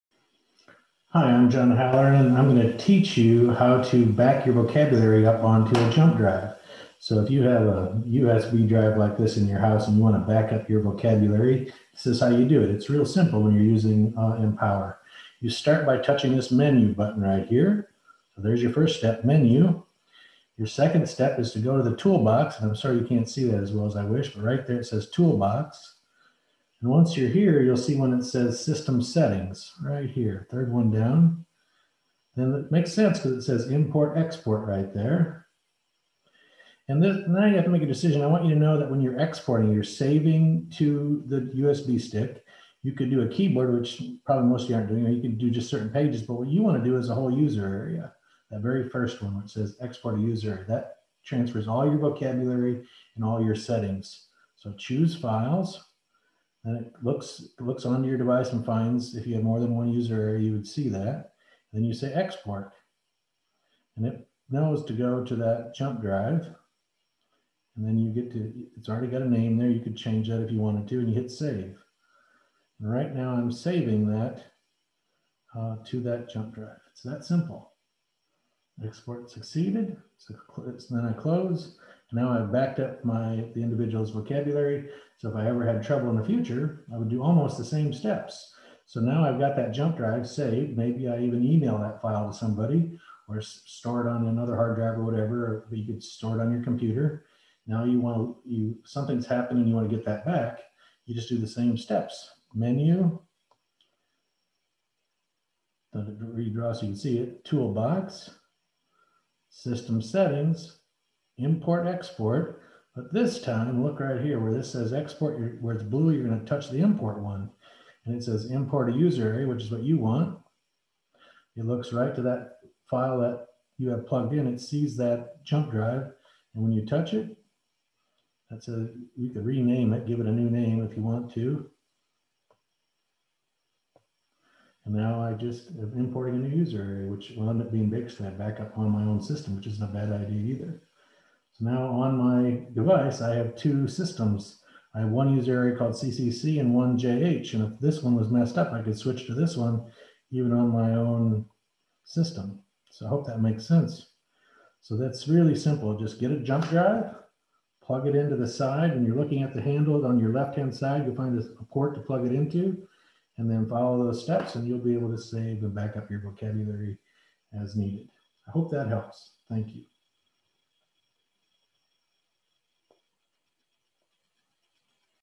Back up vocabulary using Empower on your Accent
This video teaches you how to back/export and save/import an individuals user area.